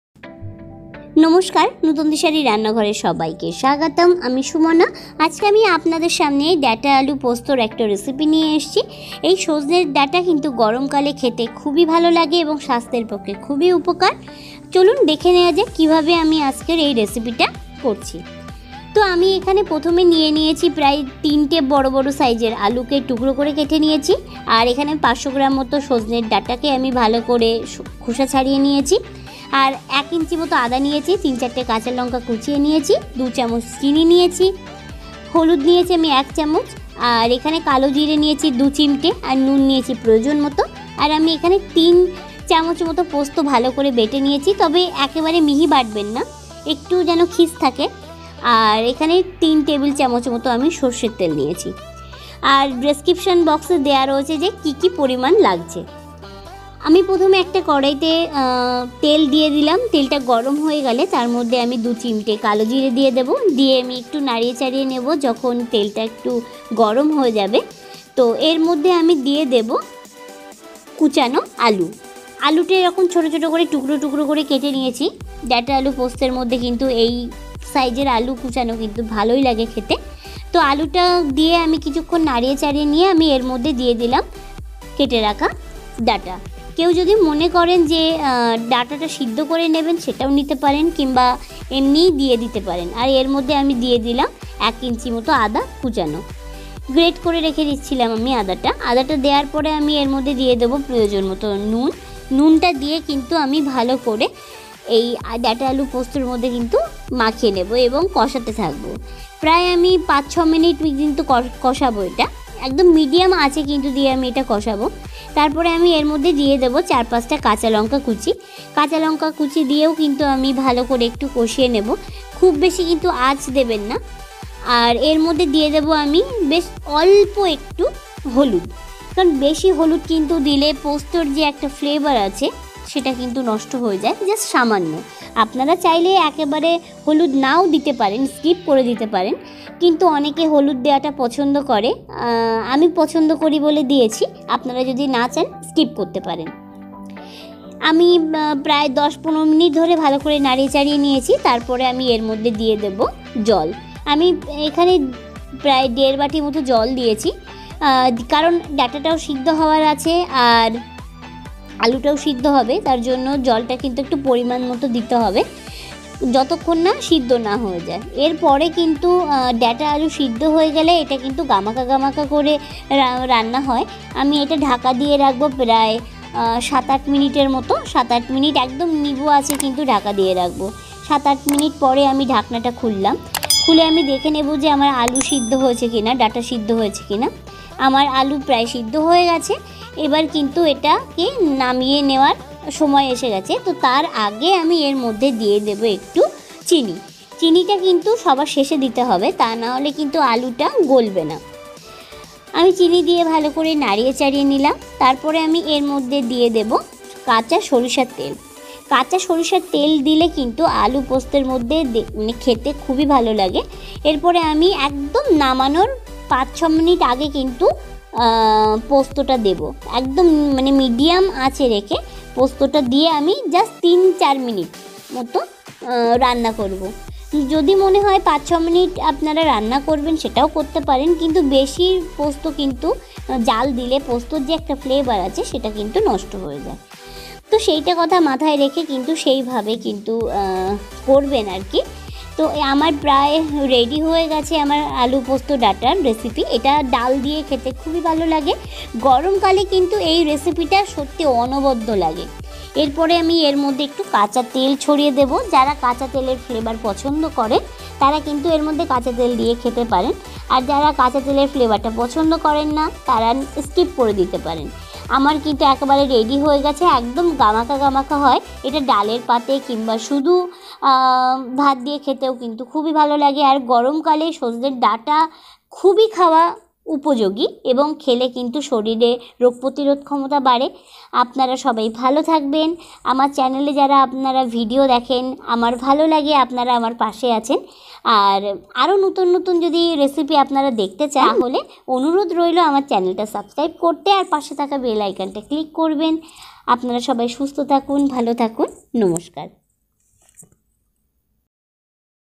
ডাটা আলু পোস্ত |গরমের উপযোগী সজনে ডাটার রেসিপি |Drumstick-Potato with Poppy | Sojne Data Aloo Posto
এই গরমে স্বাস্থ্য উপযোগী এবং এন্টিঅক্সিজেন পূর্ণ খাবার-দাবার কিন্তু খাওয়া উচিত আর গরমে বেশি তেল মশলা না খাওয়াই ভালো।
 আর সজনের ডাঁটা কিন্তু শরীরের পক্ষে খুবই উপযোগী শরীরে রোগ প্রতিরোধ ক্ষমতা বাড়িয়ে তোলে ।
  এই গরমে অনেক সময় শরীরের বিভিন্ন সমস্যা হয়, ঘা পাঁচড়া হয় তাই বেশি করে চেষ্টা করবেন যে সমস্ত তরকারির মধ্যে সজনে ডাঁটা দিতে।
  আর সজনে শুধু ডাটা নয়,
  সজনে গাছের ফুল ডাটা পাতা সবই উপযোগী আমি প্রত্যেকটা রেসিপি আমি আমার চ্যানেলে আনার চেষ্টা করবো আপনারা আমার পাশে থাকবেন আর ডাটা আলু পোস্ত কিন্তু টক ডাল দিয়ে খেতে খুবই ভালো লাগে।
   2/3রকমের টক   ডালের রেসিপি আমার চ্যানেল এ পেয়ে যাবেন।
   তবে ডাটা আলু পোস্ত গরম ভাতের সাথে কিন্তু অসাধারণ লাগে, আপনারা চেষ্টা করবেন কম তেল মসলা পুষ্টিকর খাবার খেতে আমি সবসময় চেষ্টা করি আমাদের চ্যানেলে পুষ্টিকর খাবারের রেসিপি দিতে আপনারা পাশে থাকবেন আমার আমাকে এভাবে ভালোবাসবেন ধন্যবাদ।

উপকরণ
1. সজনে ডাটা =500গ্রাম
2.আলু=2টো
3.কালো জিরে=2চিমটে
4.কাঁচ লঙ্কা=4/5ট
5.আদা বাটা=1ইঞ্চি
6.পোস্ত =3চামচ
7.লবণ= পরিমাণ মতো
8.জল=পরিমাণ মতো
9.সরিষা তেল= 3/4টেবিল চামচ
10.হলুদ=খুব সামান্য.
11.চিনি=2 চামচ